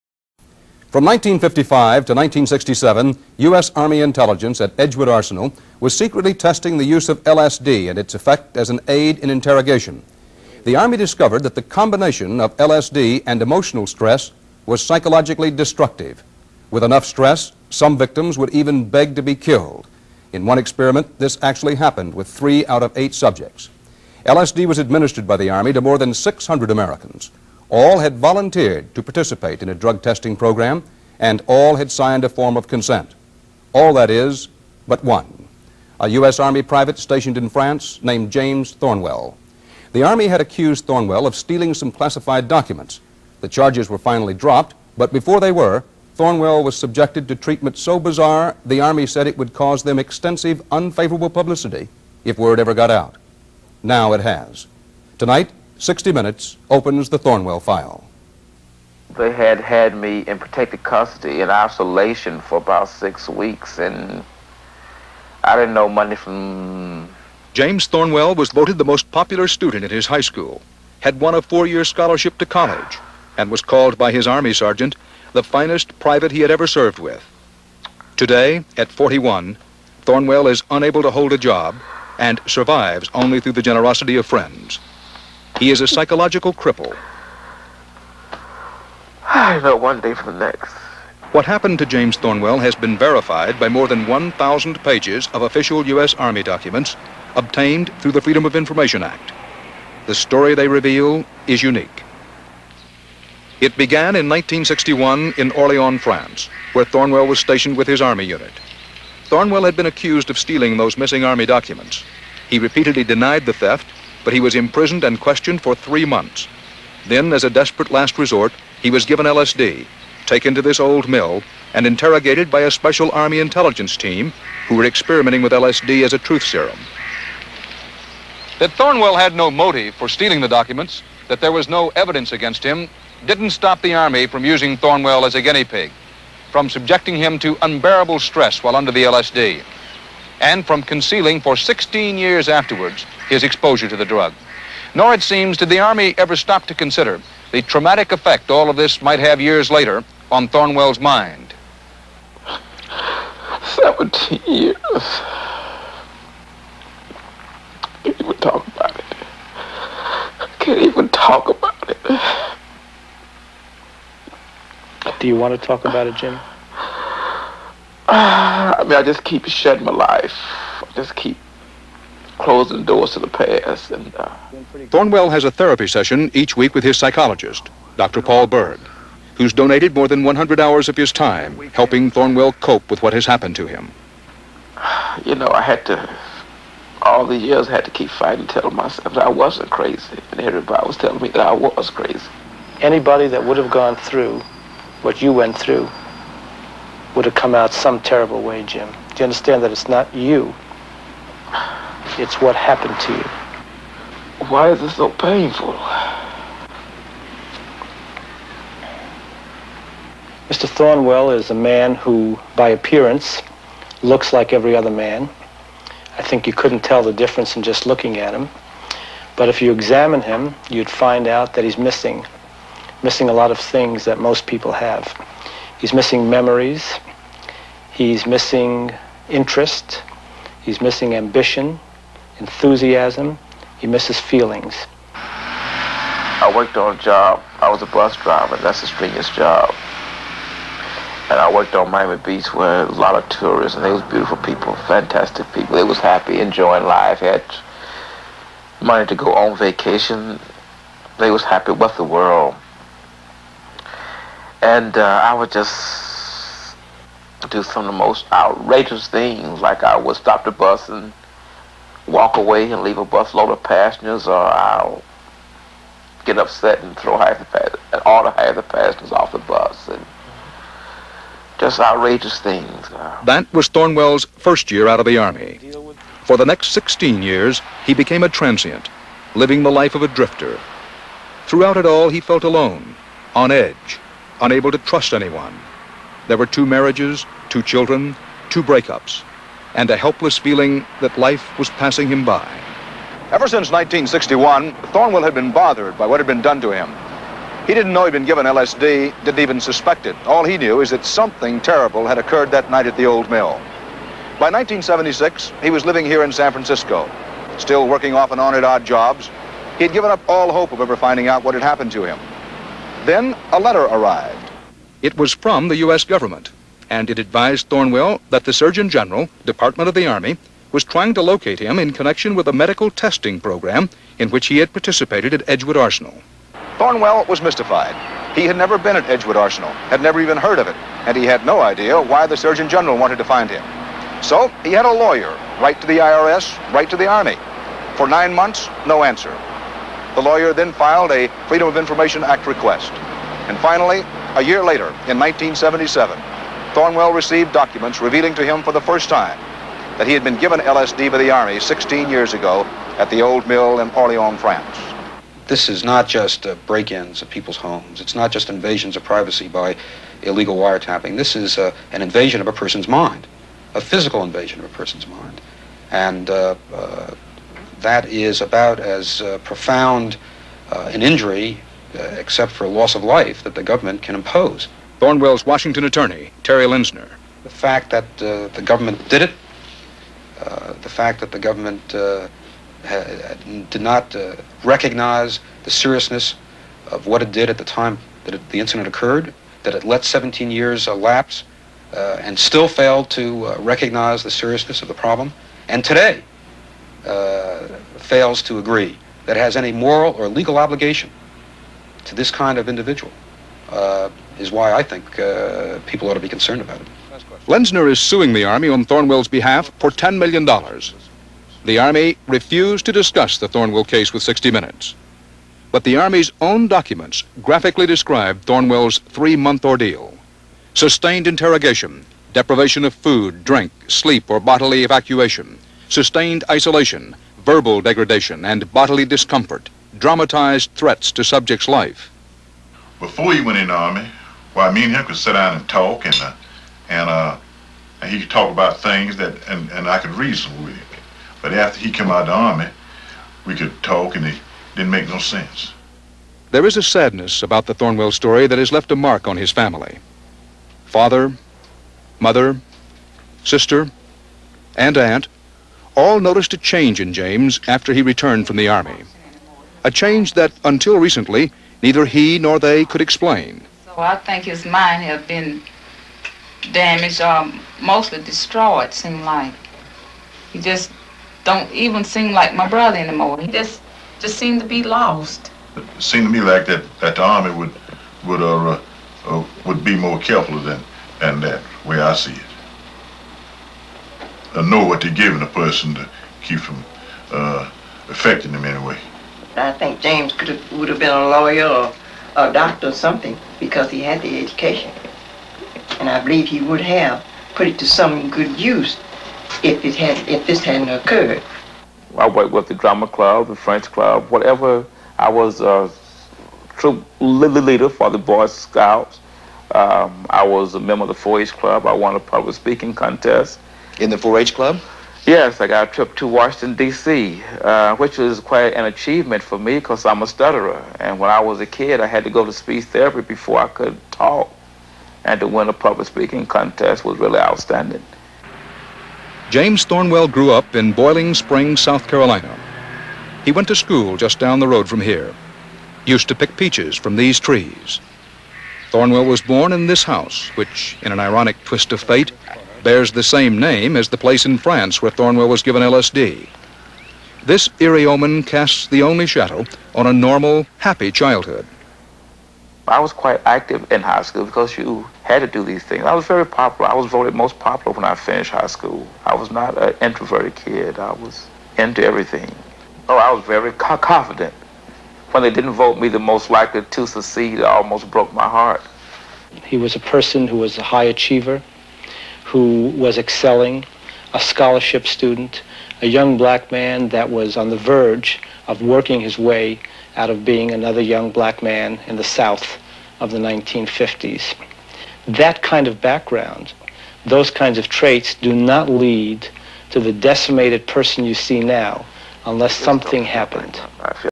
James Thornwell Part 1
Black Army soldier James Thornwell was drugged by a secret U.S. Army Intelligence unit in France in 1961, two months after Paul Robeson was also unwittingly drugged in Moscow in 1961.

Two Months after Paul Robeson died of natural causes in January 1976 in Philadelphia Pennsylvania, James Thornwell was contacted by the Surgeon General's Office in California as follow up of his 1961 LSD drugging by Army Intelligence. James Thornwell was unaware that he was drugged in 1961. In 1961 Army Intelligence was also tracking Paul Robeson and I suspect these two men were drugged in parallel, with James Thornwell as a control victim to see how the LSD drugging was affecting Paul Robeson (the intended target). Los Angeles Police Officers tell me that the timing of these two related unwitting LSD druggings are suspect. Did the United States Army coordinate the simultaneous of these two men in Europe in 1961?